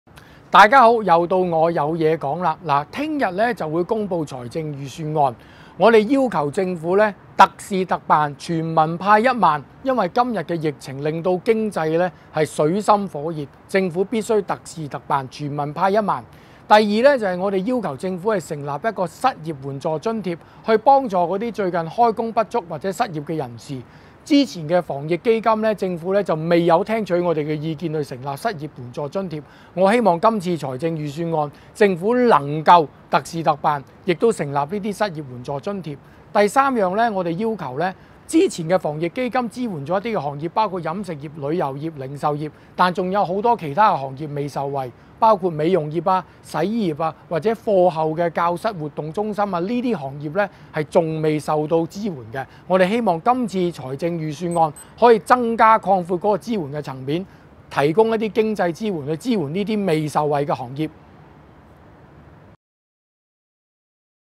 【有嘢講】民建聯周浩鼎：要求預算案設失業援助津貼(2020/02/25)
新一份預算案明天出爐，因應本港疫情肆虐，各行各行均處於水深火熱之中，我們重申，要求政府特事特辦，全民派一萬，同時要為前線職工設立失業援助津貼，以解燃眉之急。